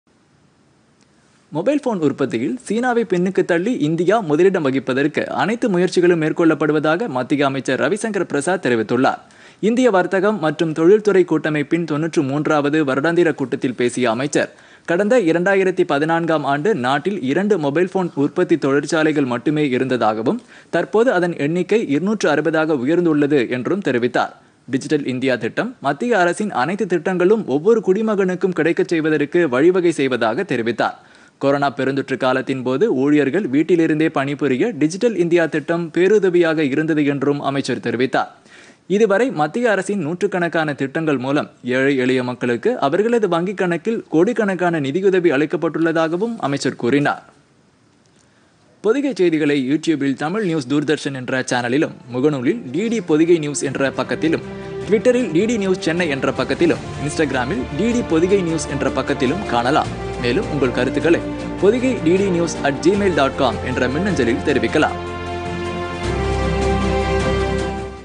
இந்தியா முதலிடம் வகிக்க நடவடிக்கை
மொபைல் போன் உற்பத்தியில் சீனாவை பின்னுக்குத் தள்ளி இந்தியா முதலிடம் வகிக்க நடவடிக்கை - மத்திய தொலைத் தொடர்புத் துறை அமைச்சர் ரவிசங்கர் பிரசாத்